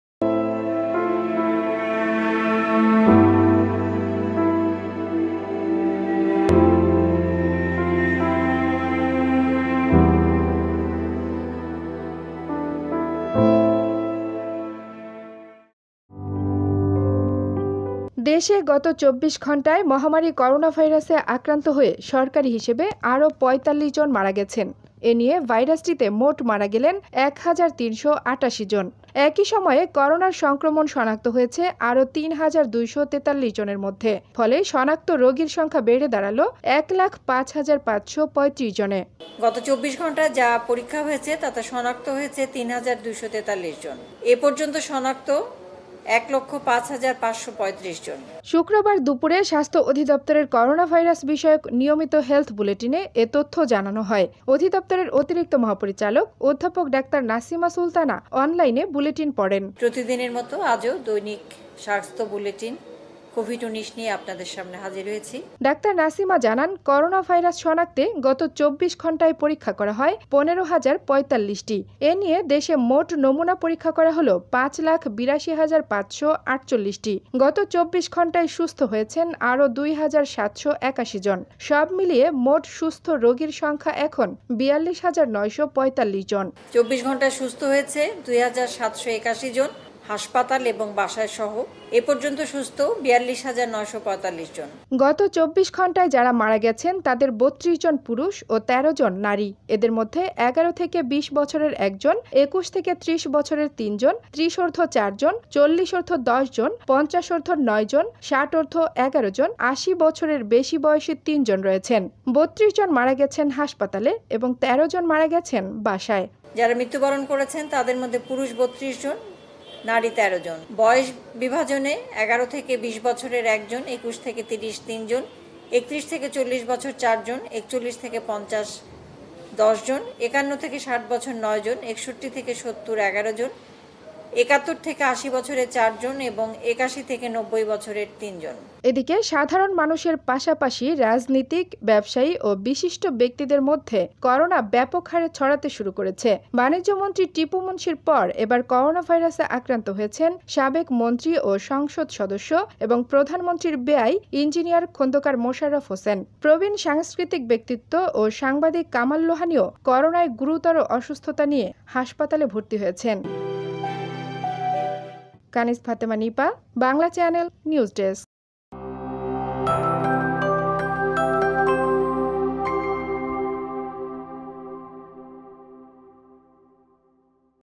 বাংলাদেশে মারা গেলেন আরও ৪৫ জন, বাণিজ্যমন্ত্রীর পর ইঞ্জিনিয়ার মোশাররফও করোনার কবলে।
দেশে গত ২৪ ঘণ্টায় মহামারি করোনাভাইরাসে আক্রান্ত হয়ে সরকারি হিসেবে আরও ৪৫ জন মারা গেছেন। এ নিয়ে ভাইরাসটিতে মোট মারা গেলেন এক হাজার ৩৮৮ জন। একই সময়ে করোনার সংক্রমণ শনাক্ত হয়েছে আরও তিন হাজার ২৪৩ জনের মধ্যে। ফলে শনাক্ত রোগীর সংখ্যা বেড়ে দাঁড়াল এক লাখ পাঁচ হাজার ৫৩৫ জনে।
এদিকে, সাধারণ মানুষের পাশাপাশি রাজনীতিক, ব্যবসায়ী ও বিশিষ্ট ব্যক্তিদের মধ্যে করোনা ব্যাপকহারে ছড়াতে শুরু করেছে। বাণিজ্যমন্ত্রী টিপু মুনসির পর এবার করোনাভাইরাসে আক্রান্ত হয়েছেন সাবেক মন্ত্রী ও সংসদ সদস্য এবং প্রধানমন্ত্রীর বেয়াই ইঞ্জিনিয়ার খন্দকার মোশাররফ হোসেন। প্রবীন সাংস্কৃতিক ব্যক্তিত্ব ও সাংবাদিক কামাল লোহানীও করোনায় গুরুতর অসুস্থতা নিয়ে হাসপাতালে ভর্তি হয়েছেন।